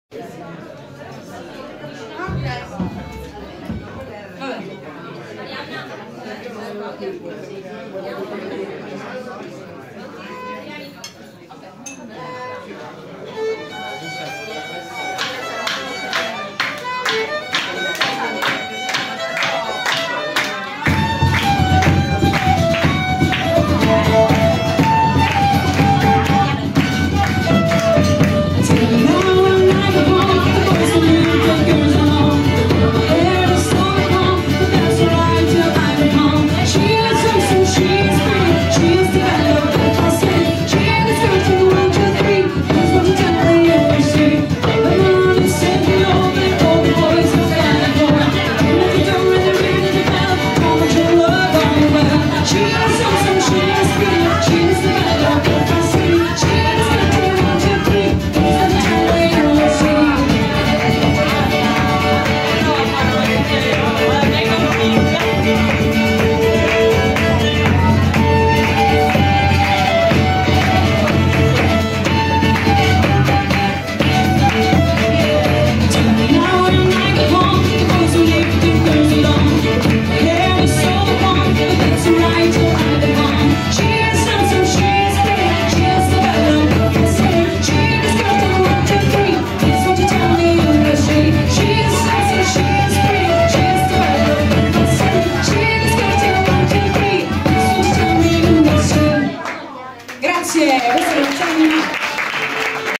The Cliffs Palermo - Tennent's Pub - Birreria Irlandese
I The Cliffs suonano al Tennent's Pub di Palermo, una birreria in vero stile irlandese, scopri dove si trova il locale, entra qui